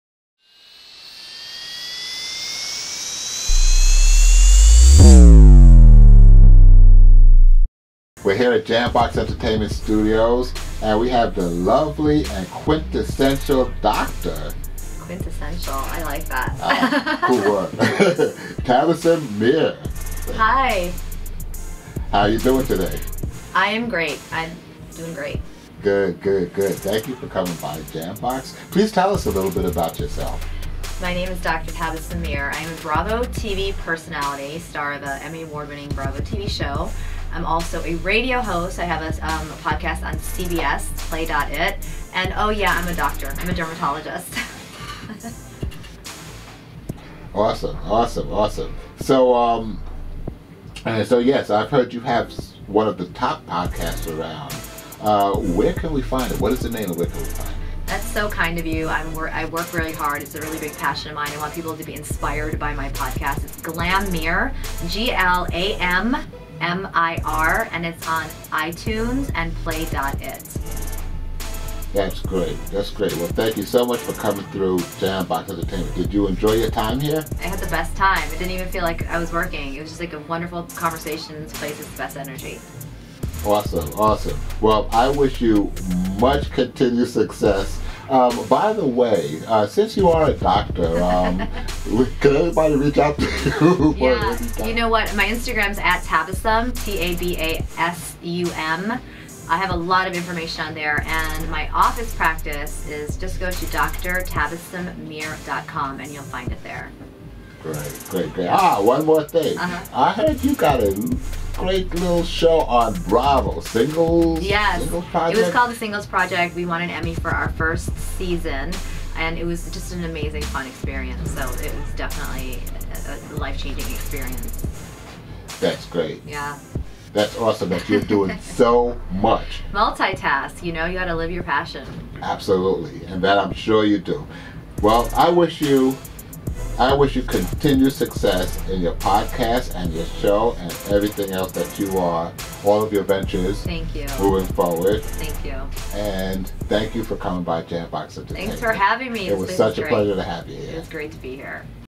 Dr Tabasum Mir Interview @ JAMBOX
Dr. Tabasum Mir from Bravo's hit series The Singles Project tells us about her experience here at JAMBOX Entertainment Studios as Alyi V interviewed her for The Alyi V Experience nofilter.